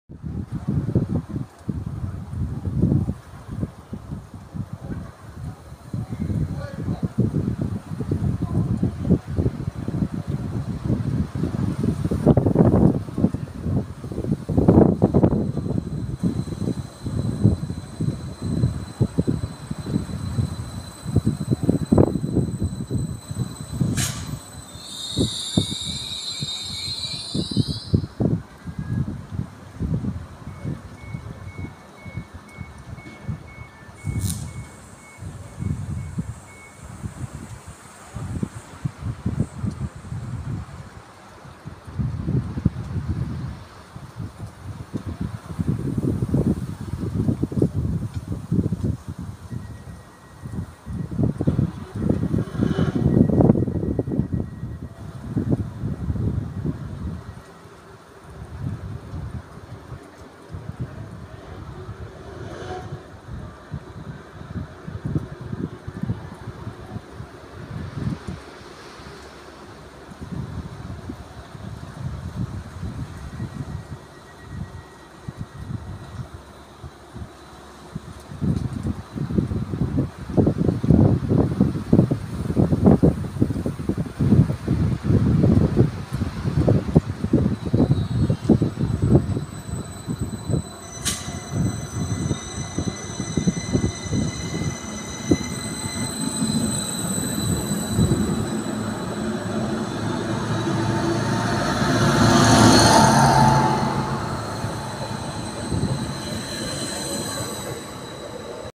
GREATER ANGLIA.755407 ARRIVING INTO STOWMARKET STATION .2023.NEIL HAYTON RAILWAY MEMORIES.
2W22 16.21 IPSWICH TO CAMBRIDGE SERVICE.